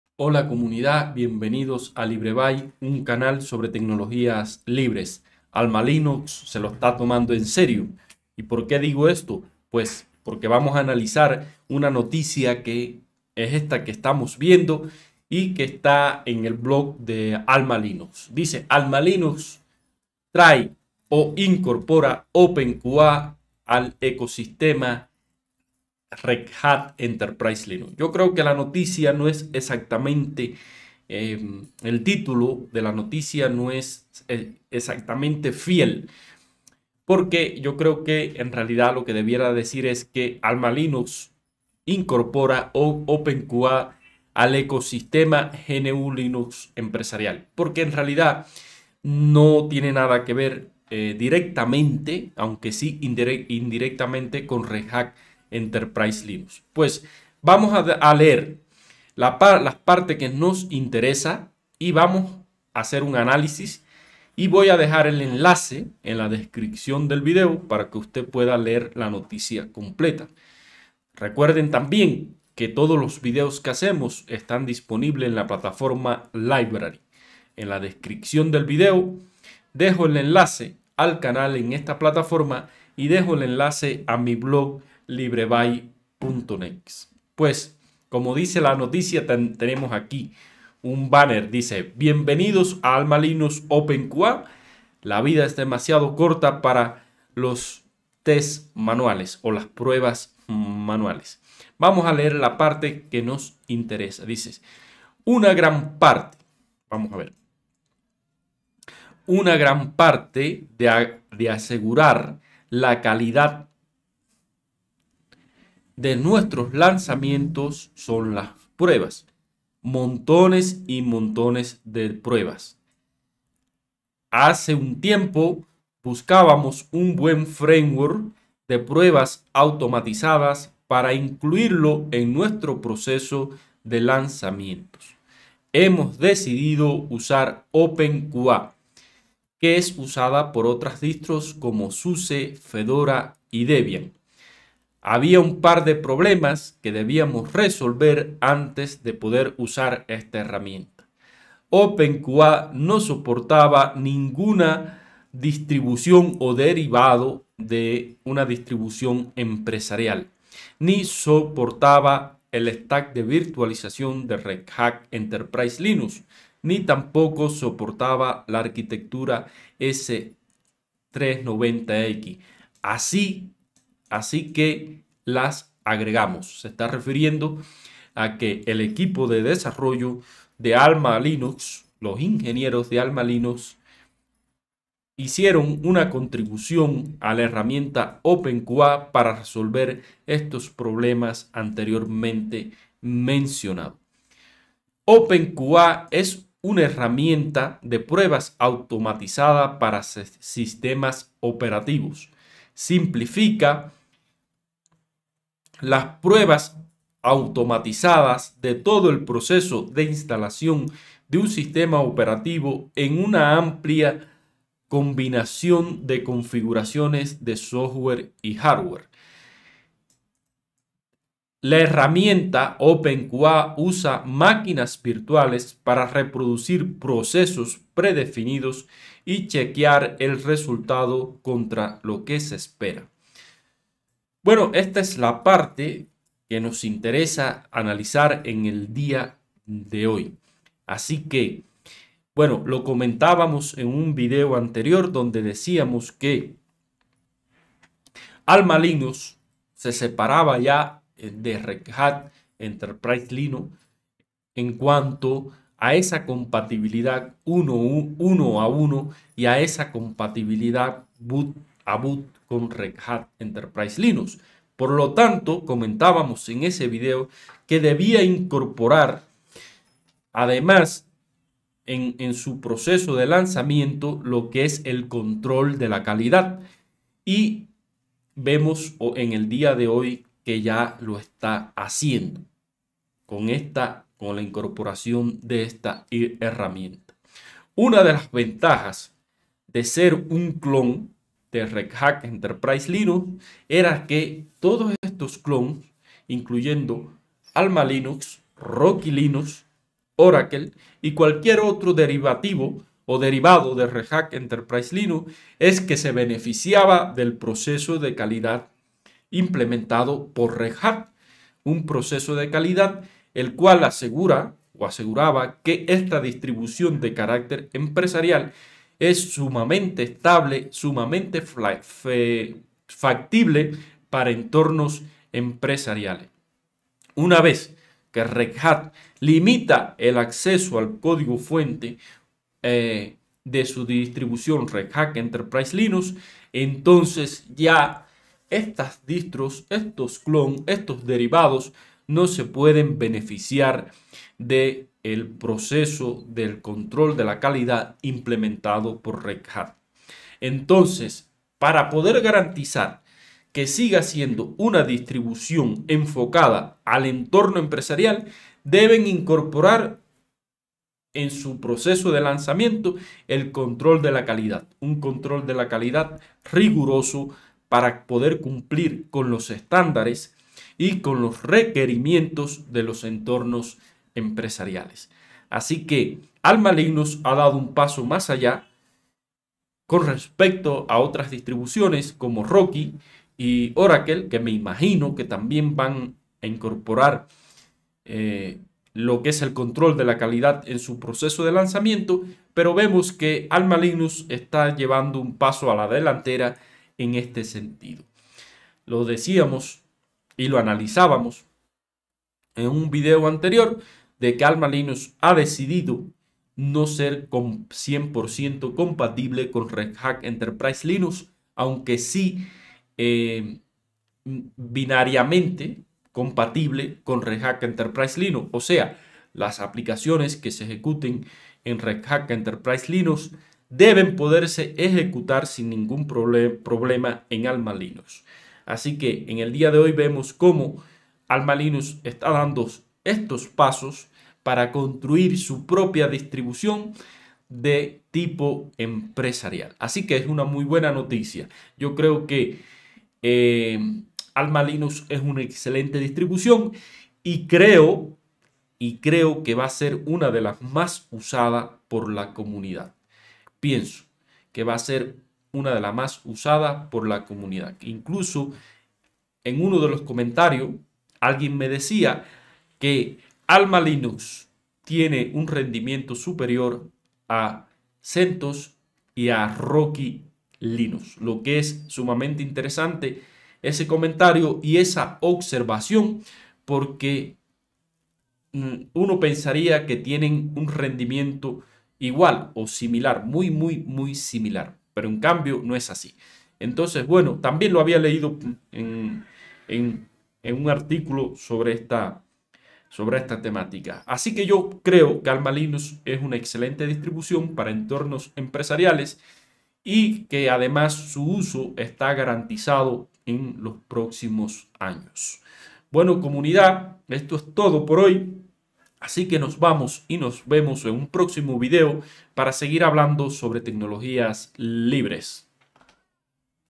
AlmaLinux se lo está tomando en serio
AlmaLinux ha decido crear su propia distro de carácter empresarial y ha comenzado a dar pasos concretos en esa dirección por eso ha incorporado openQA como parte del control de la calidad en el lanzamiento de cada versión eso y más análizamos en este video.